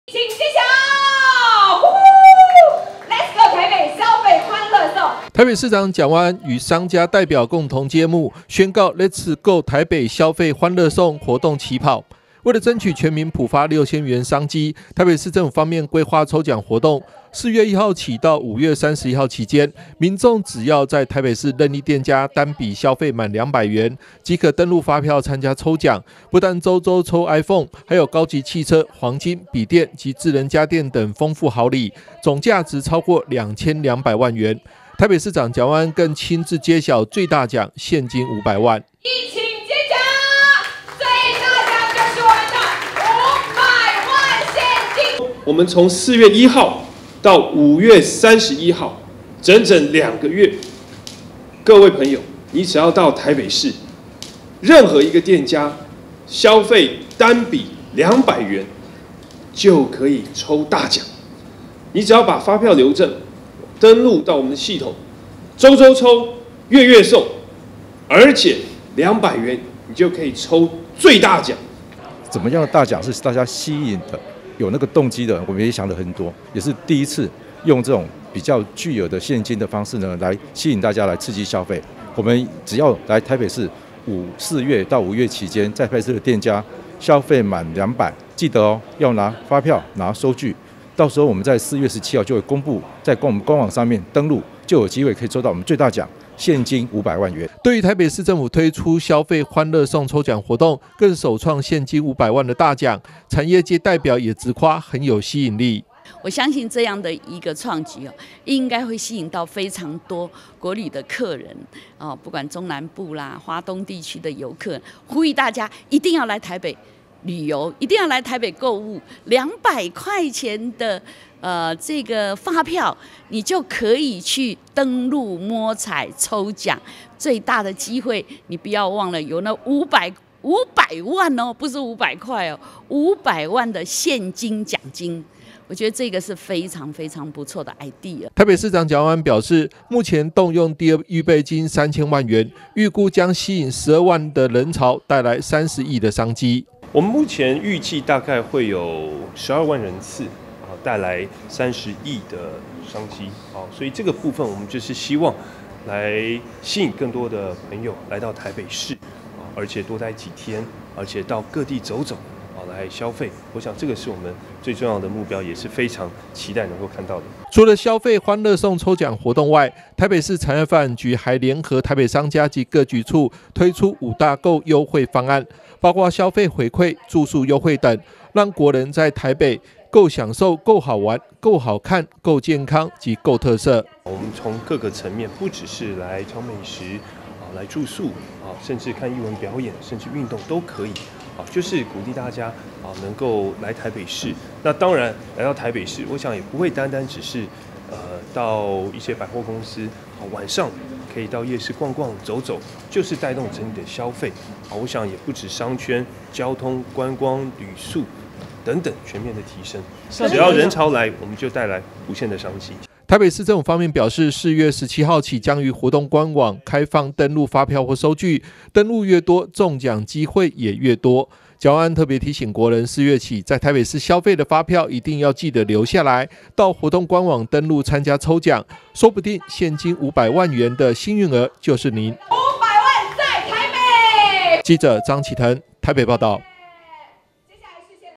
蔣萬安 五百萬|台北 歡樂購|普發 6000元|發票 抽獎|住宿 優惠|CP值 活動|2023 中嘉新聞|搶普發6000元商機 台北消費歡樂送4月1日起跑 最大獎現金五百萬
消費200元即可 蔣萬安動用第二預備金推台北歡樂購抽獎 最大獎現金五百萬

（中嘉新聞記者張啓騰/台北信義區報導）為了爭取全民普發6000元商機，台北市長蔣萬安宣布「Let’s購台北消費歡樂送」活動起跑，4/1起民眾只要在台北市任一店家單筆消費滿200元，即可憑發票登錄參加抽獎，最大獎現金五百萬元，還有高級汽車、黃金、筆電、iPhone及智能家電等獎項，總價值超過2千2百萬元，另外北市府也攜手商家推出五大「購」優惠，蔣萬安表示這次活動動用第二預備金，預估可吸引12萬人潮帶來30億元的商機。

台北市長蔣萬安與商家代表共同揭幕，宣告「Let’s購台北消費歡樂送」活動起跑，為了爭取全民普發6000元商機，台北市政府方面規劃抽獎活動，4/1起到5/31期間，民眾只要在台北市任一店家單筆消費滿200元，即可登錄發票參加抽獎，不但周周抽iPhone，還有高級汽車、黃金、筆電及智能家電等豐富好禮，總價值超過2200萬元，台北市長蔣萬安更親自揭曉最大獎-現金五百萬。

對於台北市政府推出消費歡樂送抽獎活動，更首創現金五百萬的大獎，產業界代表也直誇很有吸引力。

台北市長蔣萬安表示目前動用第二預備金3000萬元，預估將吸引12萬的人潮帶來30億商機。

除了消費歡樂送抽獎活動外，台北市產業發展局還聯合台北商家及各局處推出五大購優惠方案，包括消費回饋、住宿優惠等，讓國人在台北購饗受、購好玩、購好看、購健康及購特色。

台北市政府方面表示4/17起將於活動官網開放登錄發票或收據，登錄越多中獎機會也越多，蔣萬安特別提醒國人4月起在台北市消費的發票一定要記得留下來，到活動官網登錄參加抽獎，說不定現金五百萬的幸運兒就是你。